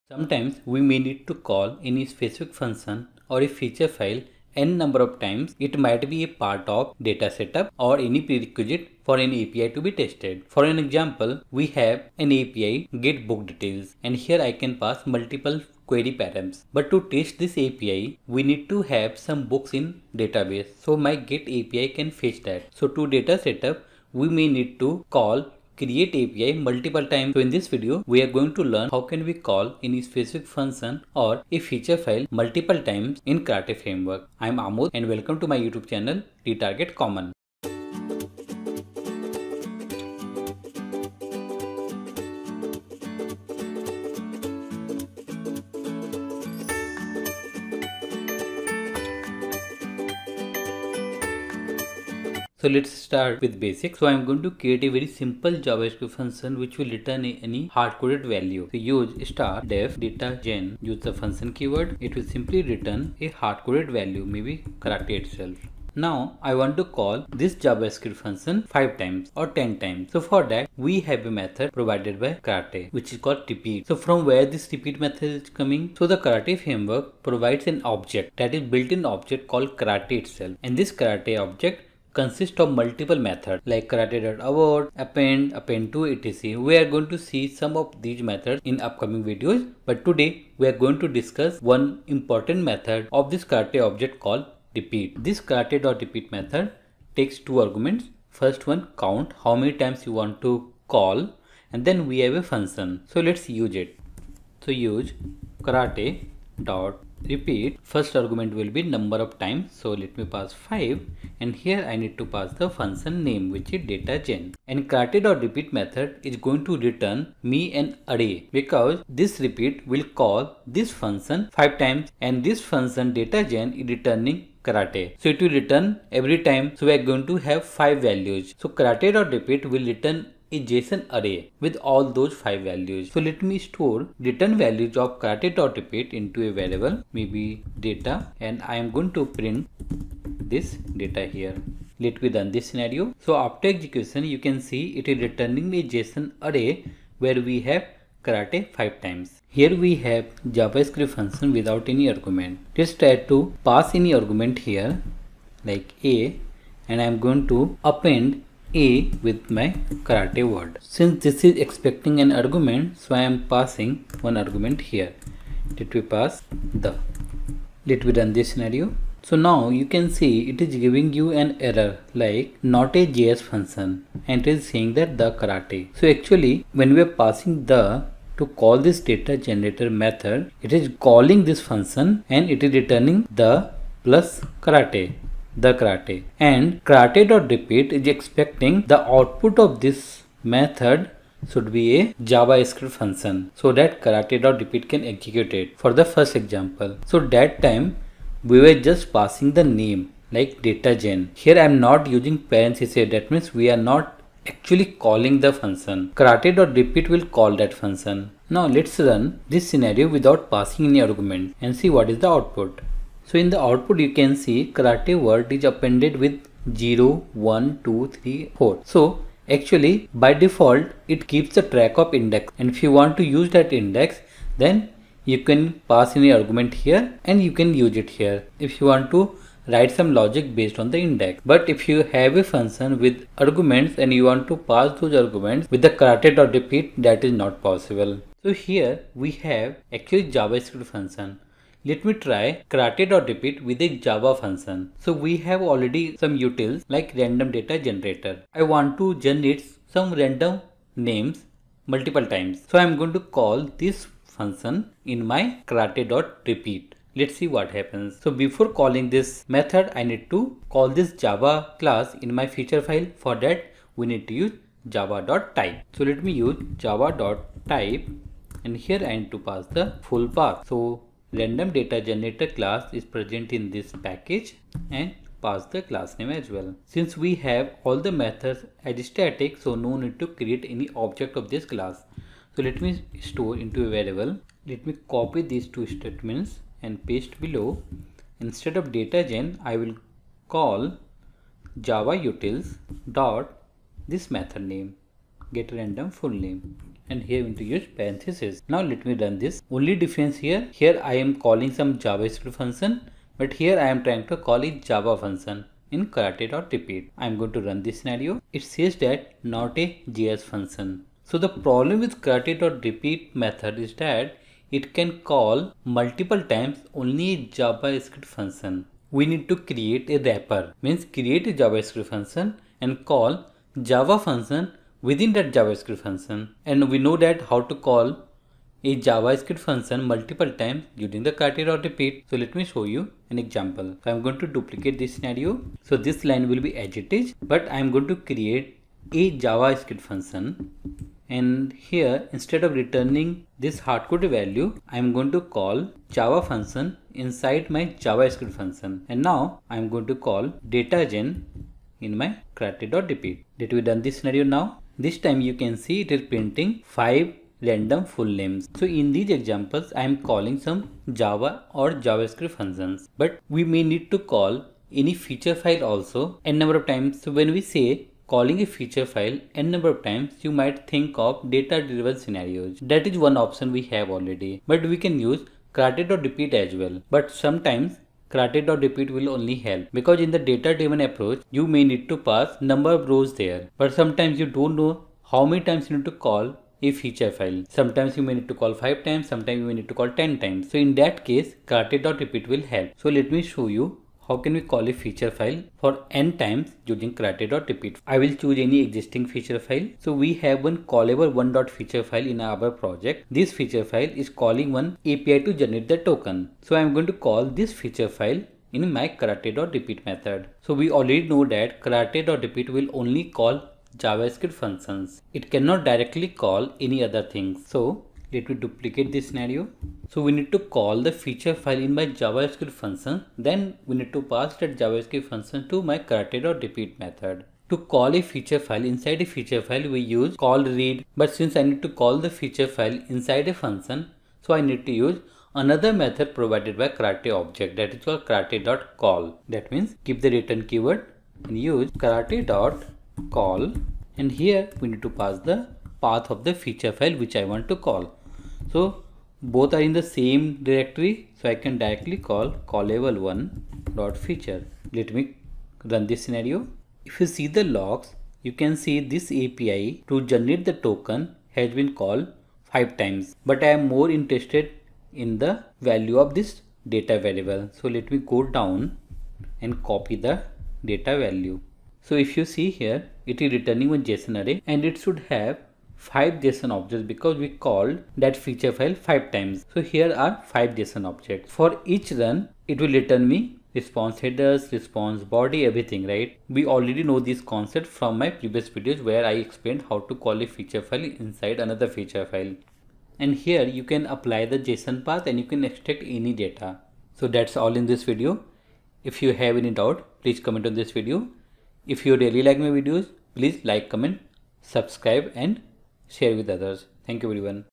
#22. Karate Repeat Method - Call JS Function & Feature File For N Times
Is my YouTube channel helping you?
If No, feel free to drop a comment/email.

It is possible to call JavaScript, Java functions, and feature files multiple times or N times using karate.repeat() method. Let's learn more about this in this video -